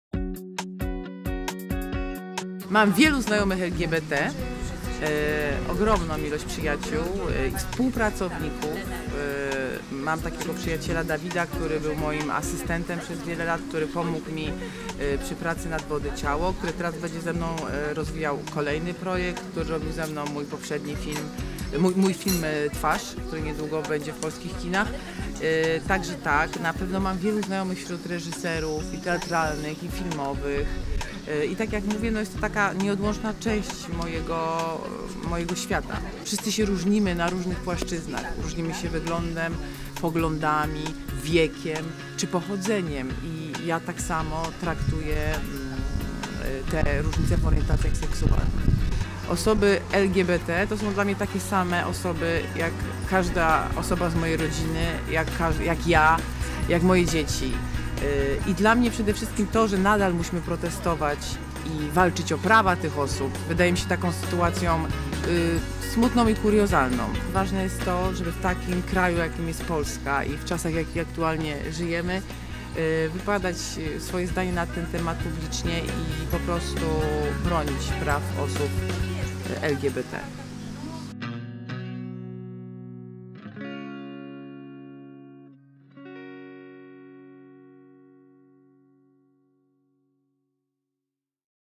Małgorzata Szumowska kolejną sojuszniczką osób LGBT
Małgorzata Szumowska, reżyserka i scenarzystka, kolejną ambasadorką akcji "Ramię w ramię po równość"
Obejrzyj filmik, w którym Szumowska mówi o tym jaka jest rola sojuszników i sojuszniczek osób LGBT.
fot. Agata Kubis wideo Michal Henzler